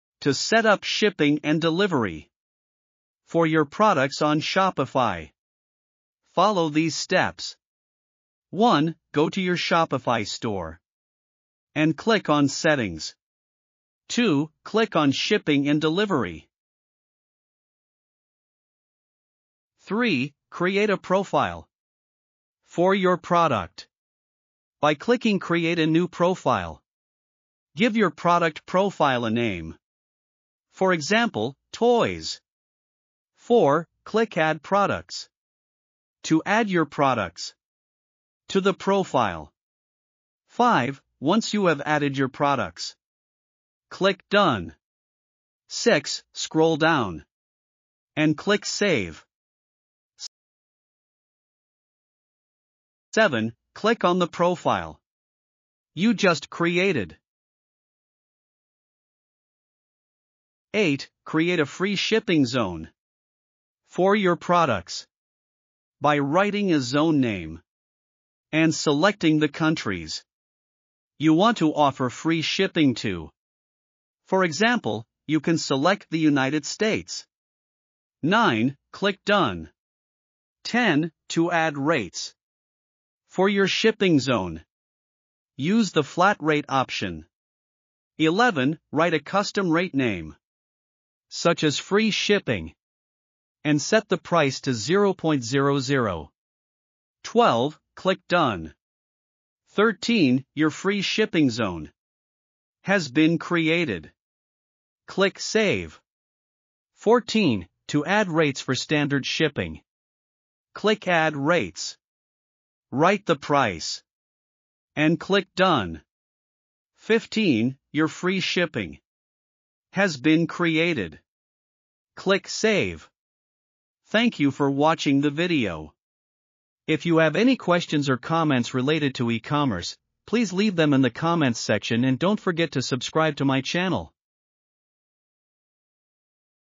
How to Set up Free Shipping on Shopify 2024 (Step-by-Step)
In this video I’ll show you how to set up free shipping on shopify!